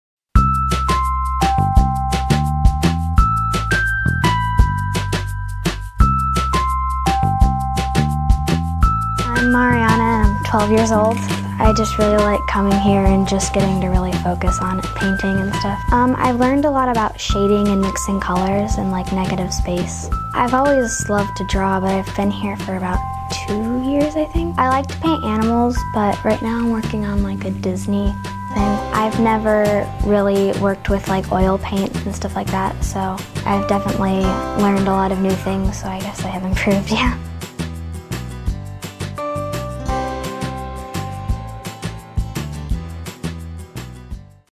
Student Testimonial: Mariana | Martin Fine Art Classes
Mariana attends Martin Fine Art Classes because she learns how to make her paintings more realistic. We are located in Encinitas, and we offer painting instructions in San Diego’s North County area for either children, teens or adults.